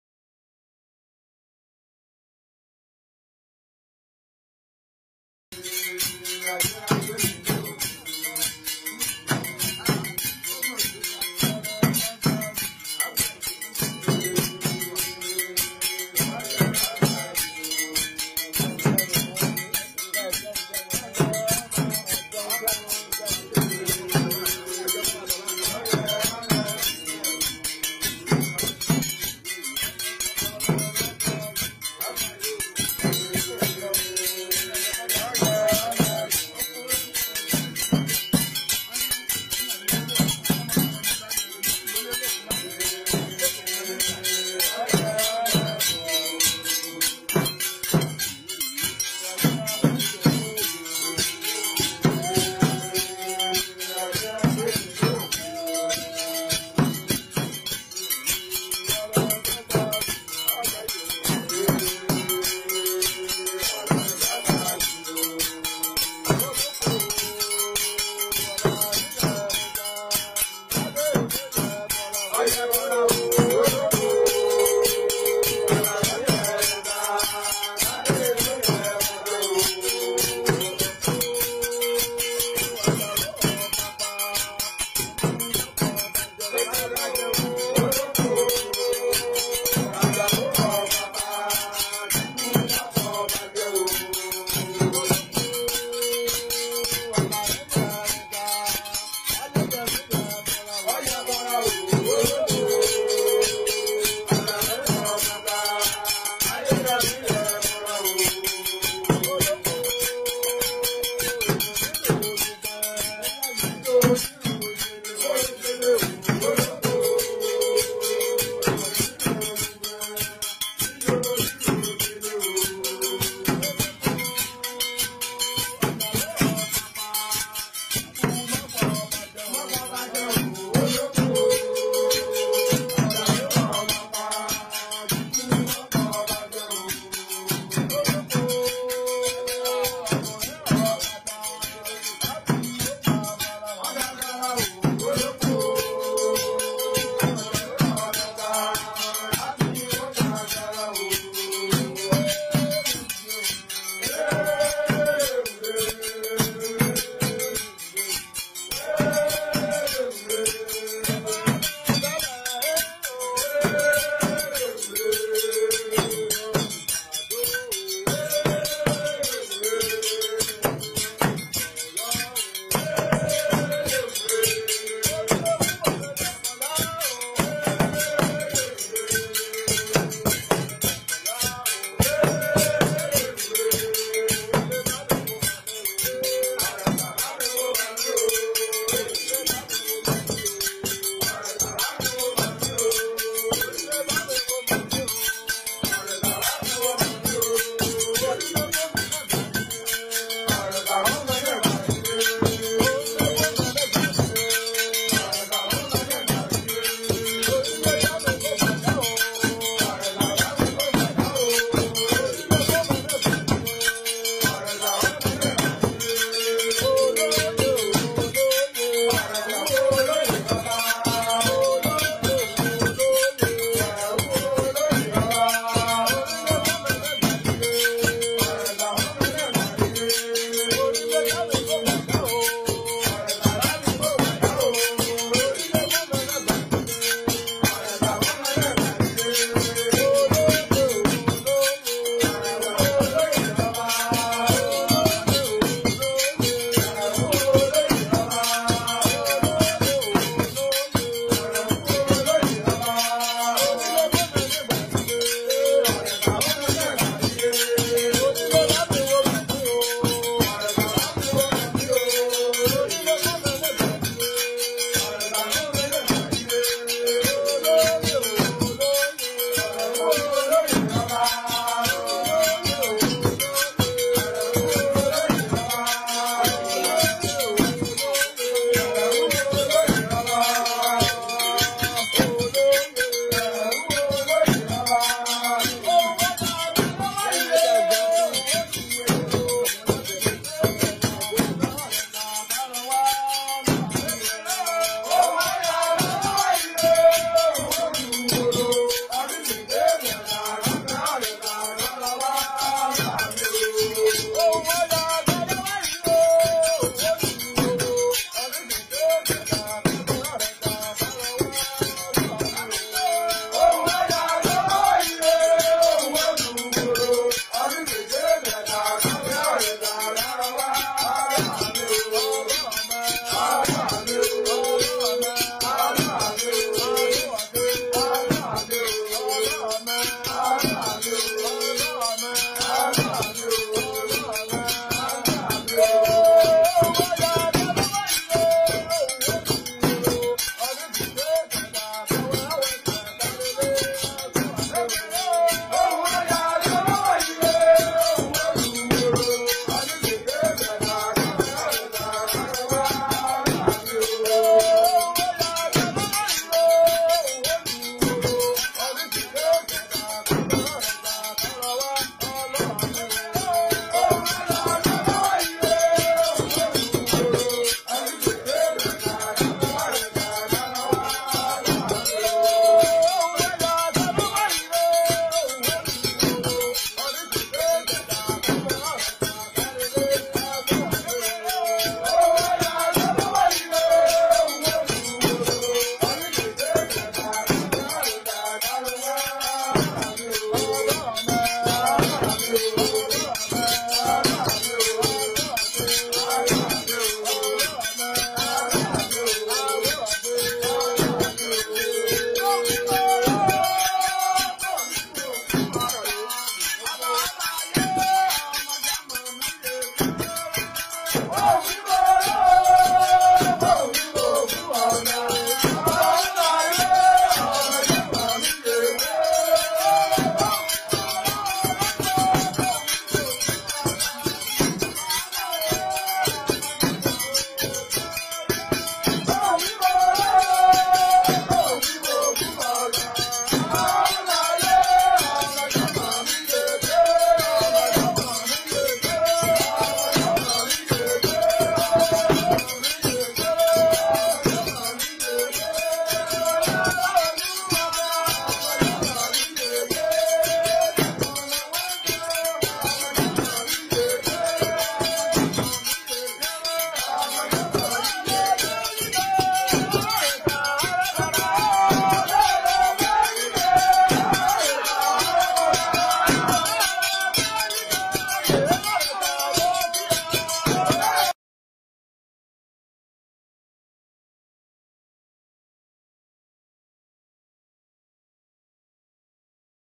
Oxalá Templo nigeriano, em Ilè Ifé. Obatalá Temple.
AJUDA EM NOSSA VAKINHA - Ação social e Caridade

Cantigas de Oxalá - Orins Oxalá. Candomblé Ketú
Cantigas de Òbáátálá, Orixá Funfun, Oxaguian e Oxalufan.

Adquira nossas Apostilas e Livros e muito mais artigos religiosos.

Axé o !